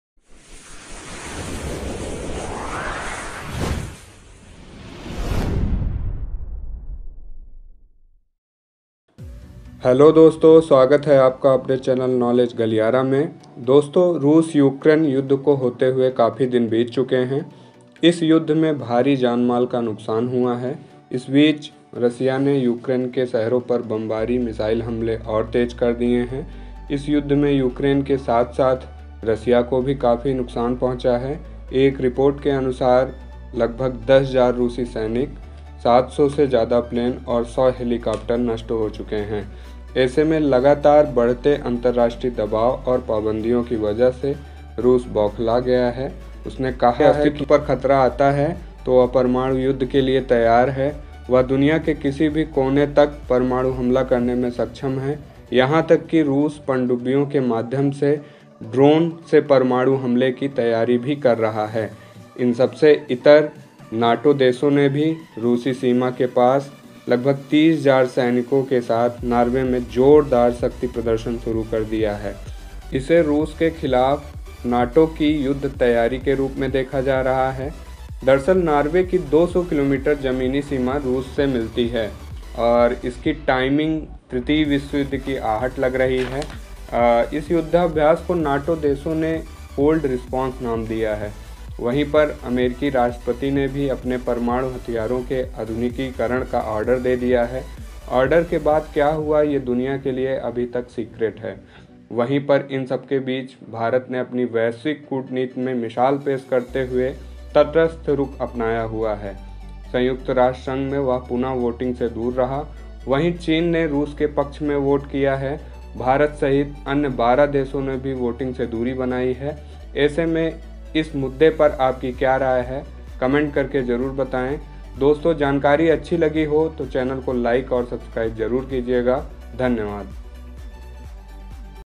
नाटों ने हद कर दी।अब विश्वयुद्ध होकर रहेगा।russia ukraine war.रूस यूक्रेन युद्ध।full video in hindi.
about this video-:
 dosto is video me aapko russia ukraine yuddh k bare me btaya gya hai. sath hi sath narway me chal rhi nato desho ki exercise ko bhi describe kiya gya h..russia ukraine yuddh me bharat ke stand ki bhi charcha ki gyi h.

Queries solved:-
1). russia ukraine war.
2).india's stand on russia-ukraine war.
3).united nation voting.
4).nato exercise.